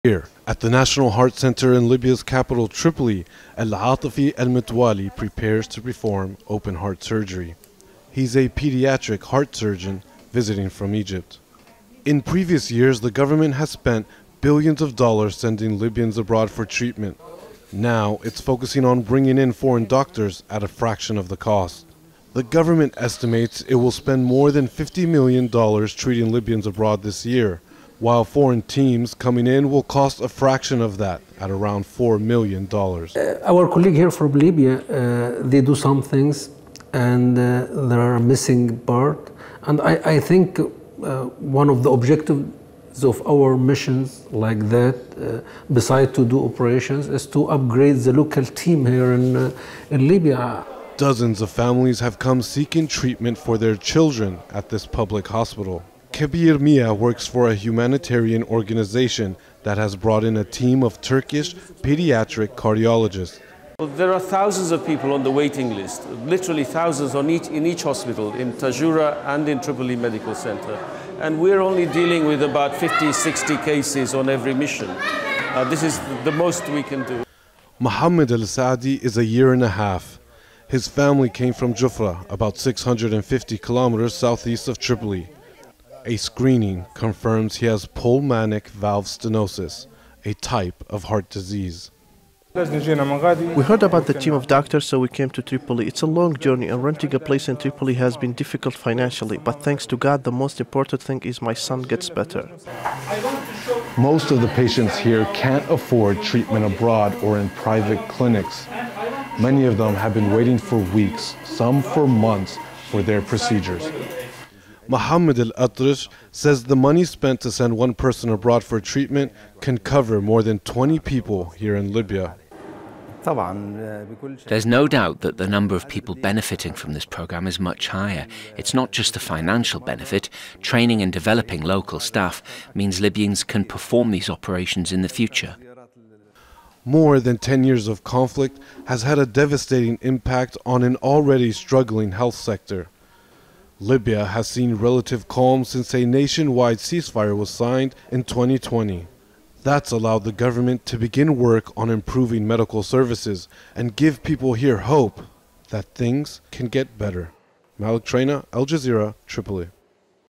Tripoli bringing in foreign doctors to improve Libya's healthcare
Libya's Tripoli-based government is working to improve the country's healthcare services after a decade of conflict left the sector on the verge of collapse.

One of its goals is to stop sending patients abroad and bring in foreign doctors to help fill the gap.

Al Jazeera's Malik Trainia reports from a medical centre in Tripoli, Libya.